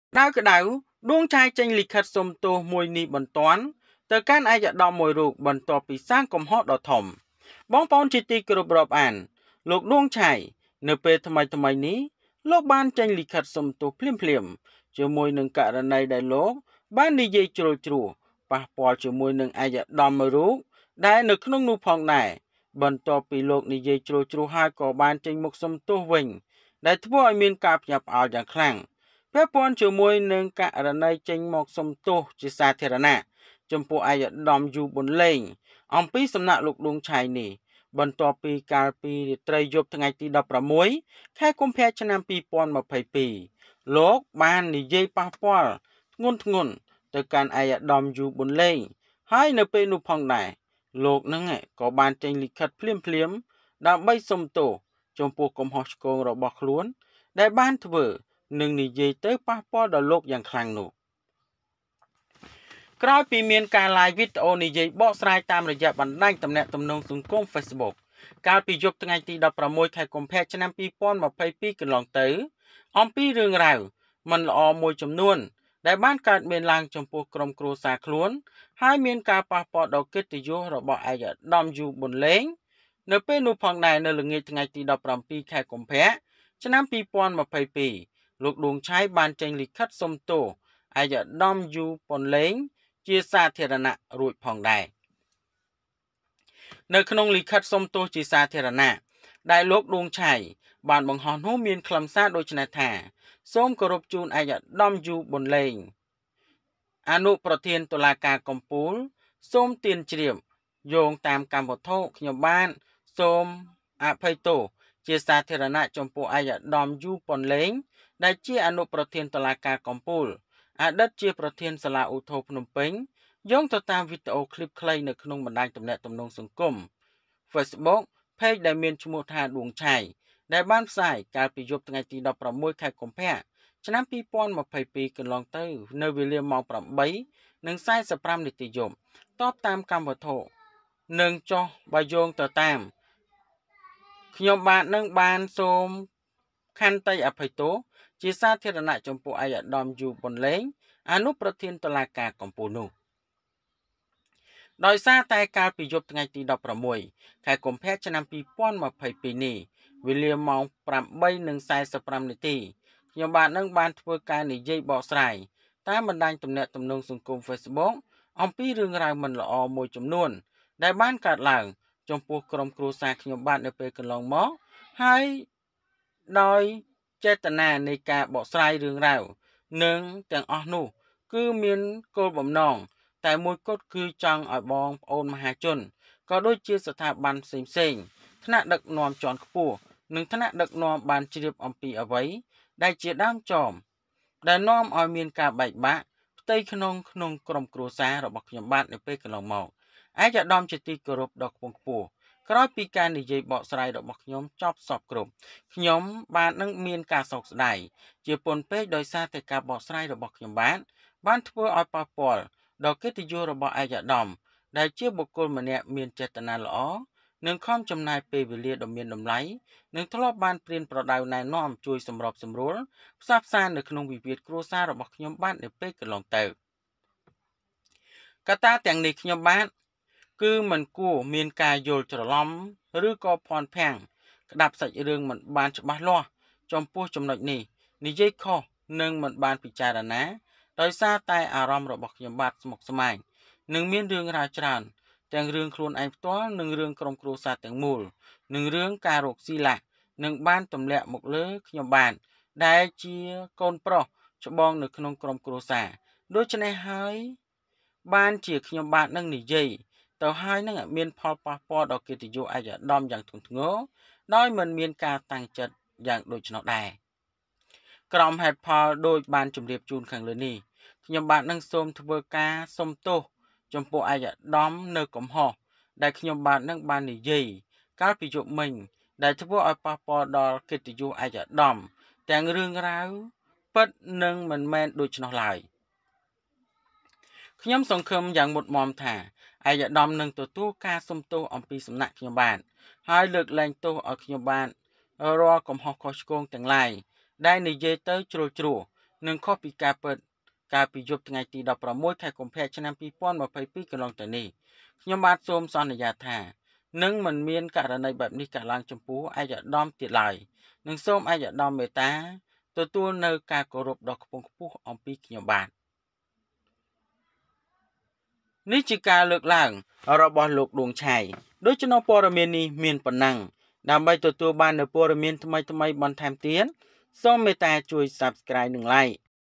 ក្តៅៗ ឌួង ឆាយ ចេញលិខិតសុំទោសនេះ បន្ទាន់ ទៅកាន់ ​ឯកឧត្ដមមួយរូបនេះ បន្ទាន់ ព្រោះតែសាងកំហុសធំនេះ
ក្តៅៗ ឌួង ឆាយ ចេញលិខិតសុំទោសនេះ បន្ទាន់ ទៅកាន់ ​ឯកឧត្ដមមួយរូបនេះ បន្ទាន់ ព្រោះតែសាងកំហុសធំនេះ Duong Chhay issued this apology letter to His Excellency immediately because of this big mistake.